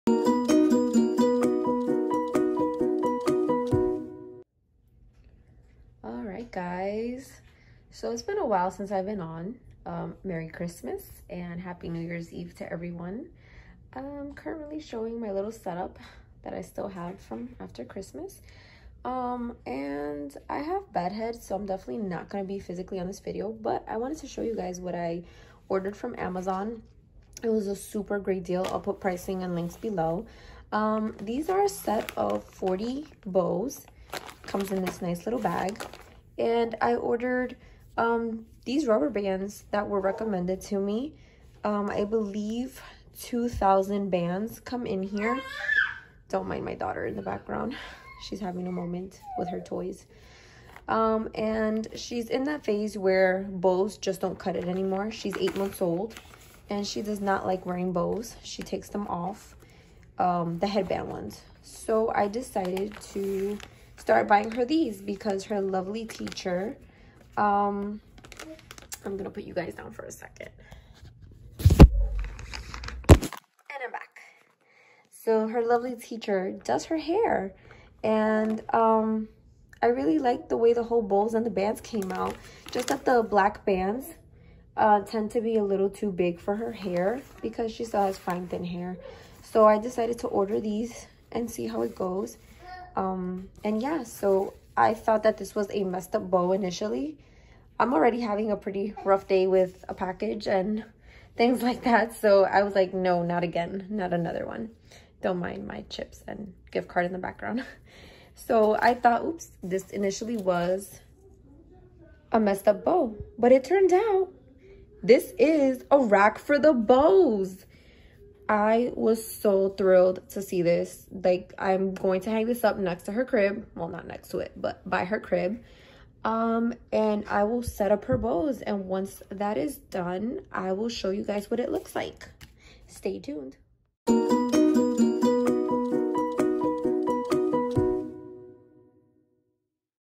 Deals on Amazon: Hair Bows and hair ties
Being a mom to a beautiful 8 month old with hair growing in pretty fast, I am more than excited to start putting clip bows on her hair! I’ve been searching around for some time and I love a good Amazon deal/find. Check it out!

40 piece bow set for $6.99⬇️

Oaoleer 40pcs 3 Inches hair bows...

Plastic band ties for $4.99⬇️

EAONE 2000 Pieces Multi-color...